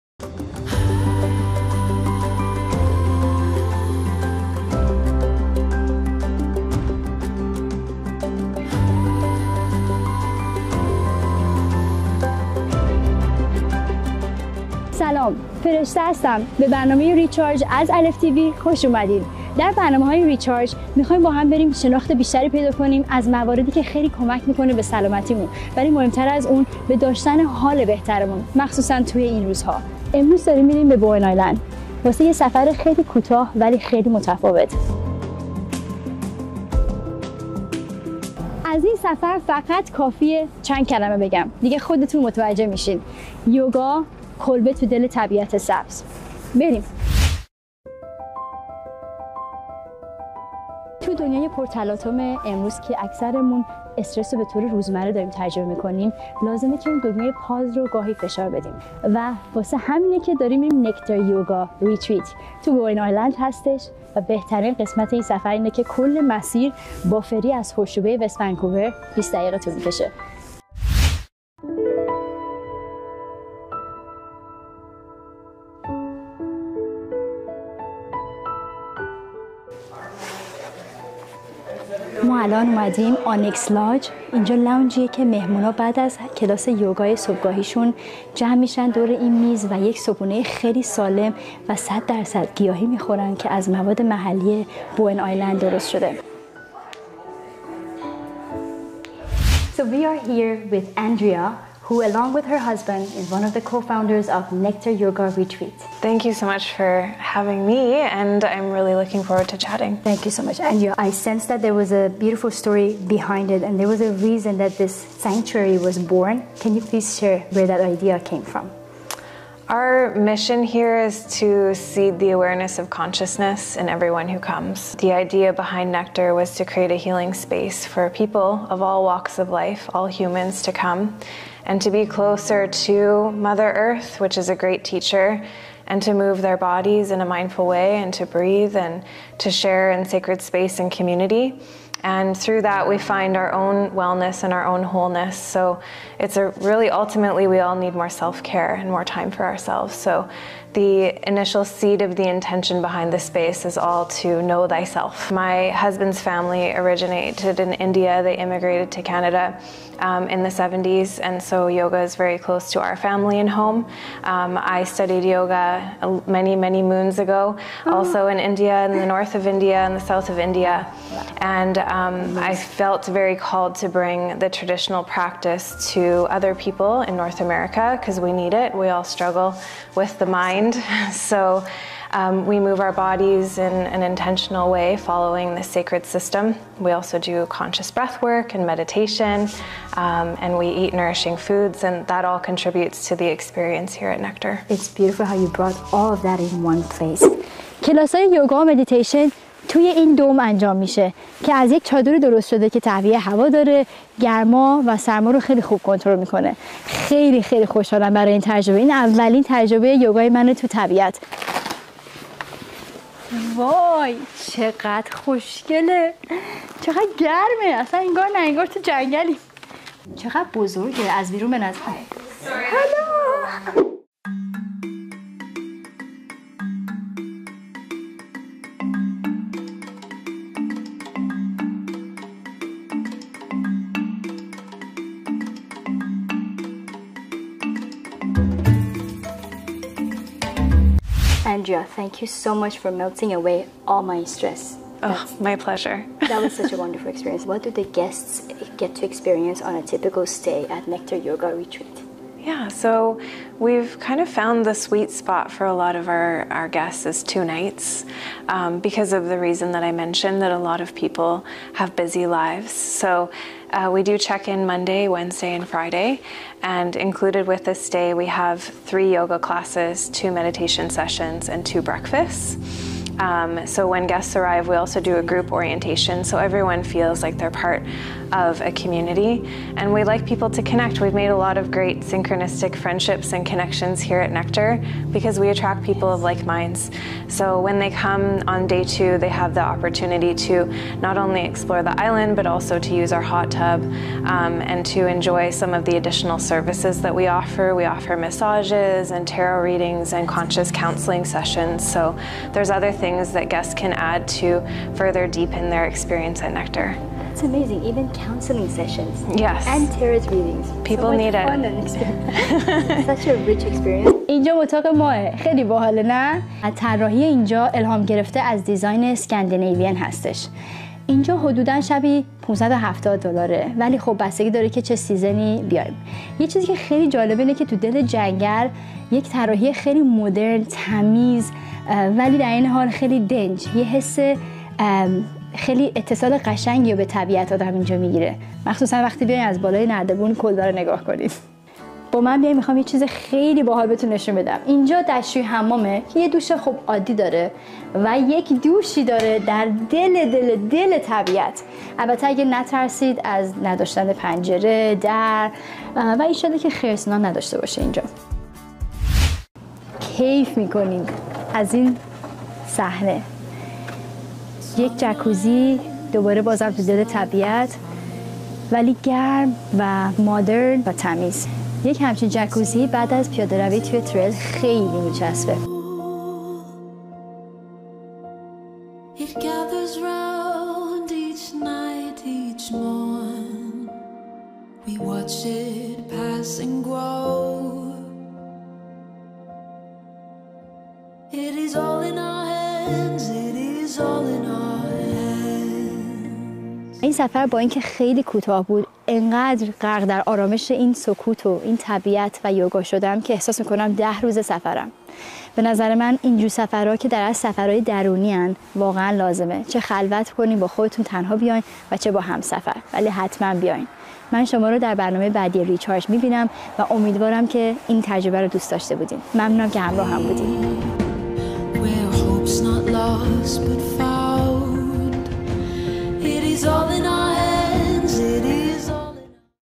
ALEPH Exploring Nectar Yoga
Recently we had a chance to visit one-of-a-kind Yoga retreats located in Bowen island, Nectar Yoga. Join us as we enjoy the beautiful scenery of the island, while relaxing through yoga and meditation. We also managed to have a chat with one of the owners of Nectar yoga to learn more about the intention behind establishing this retreat.
We encourage everyone to try this unique experience.

Hope you enjoy watching this episode!

Special thanks to:
Nectar Yoga
Host: fereshteh.khm
fereshteh.khm